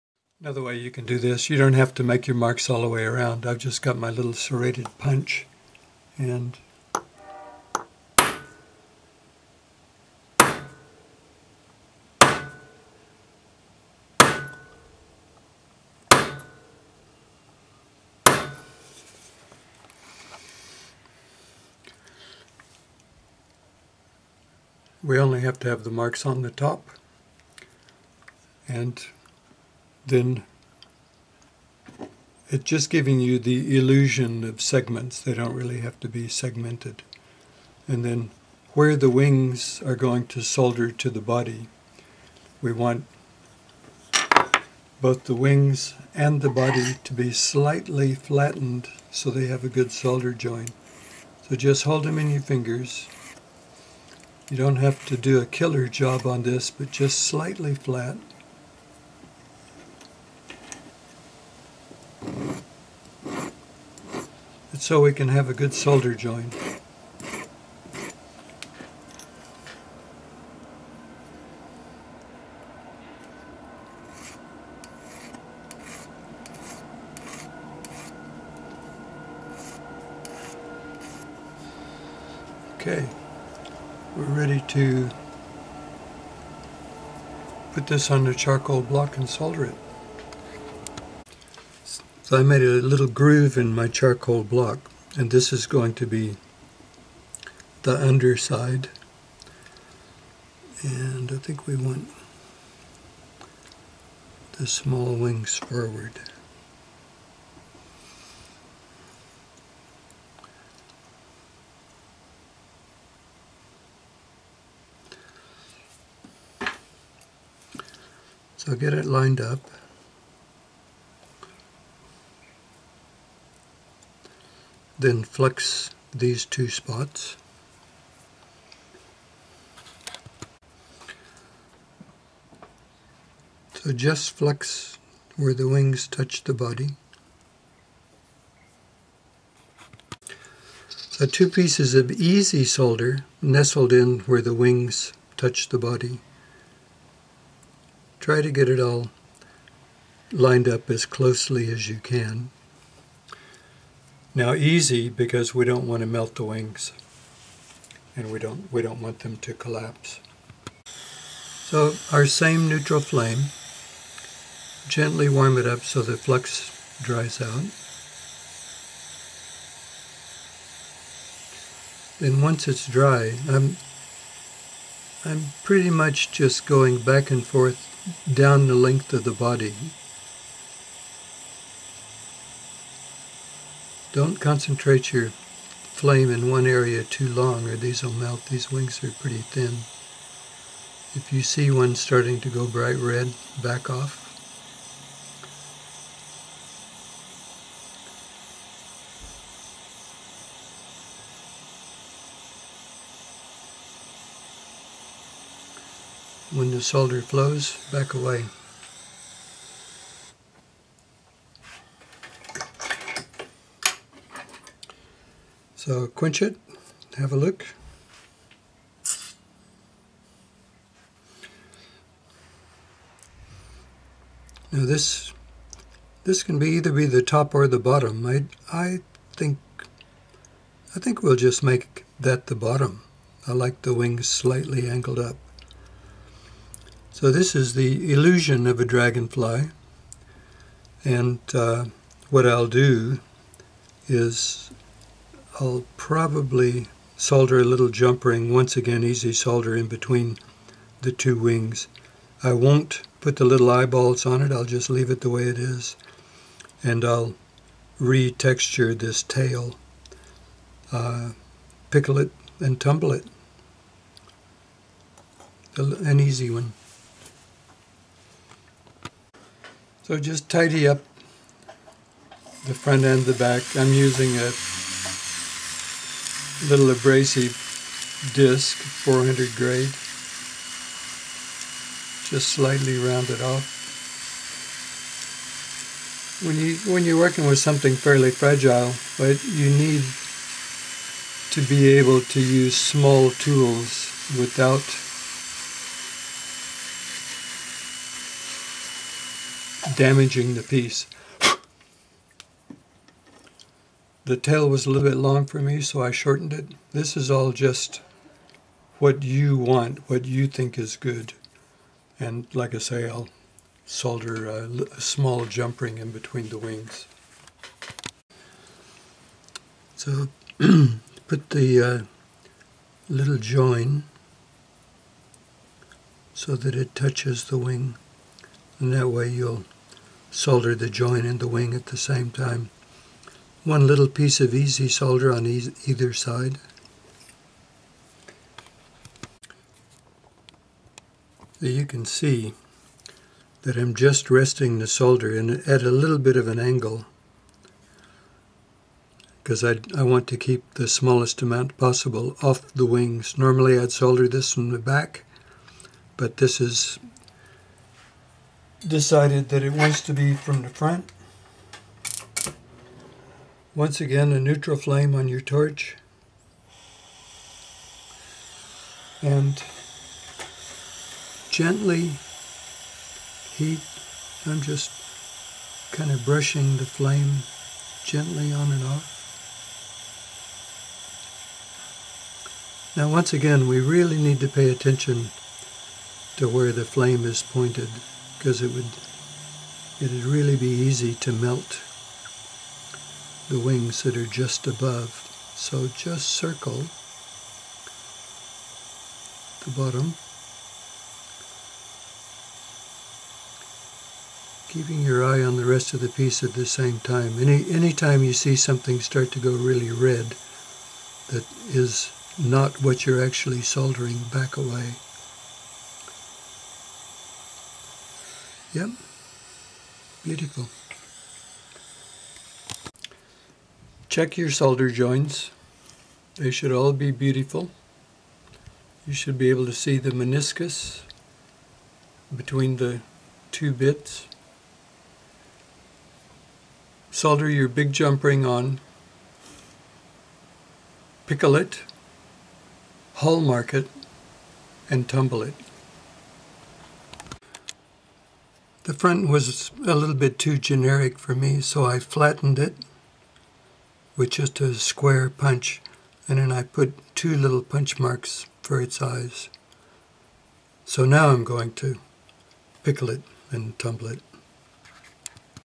#2 dragonfly part 2
making a silver dragonfly pendant